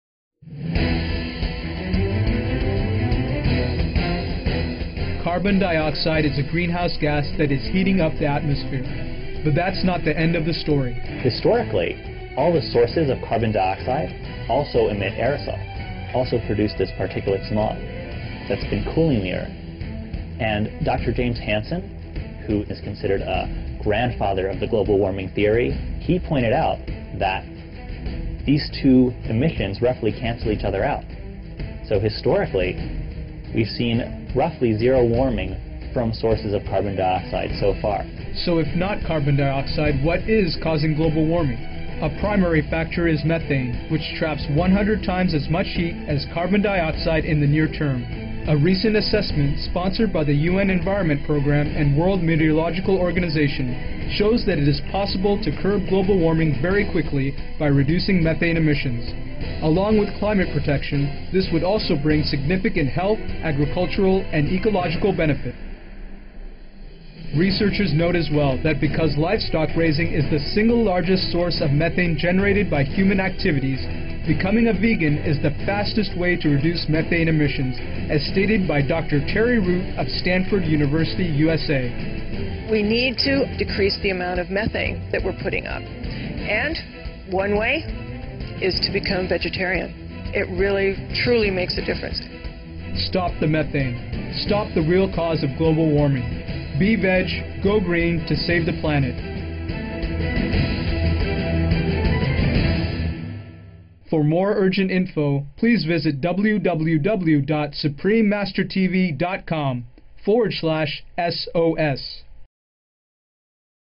무엇이 지구온난화를 야기할가요?
메탄을 막읍시다! 지구온난화의 진정한 원인을 막읍시다!!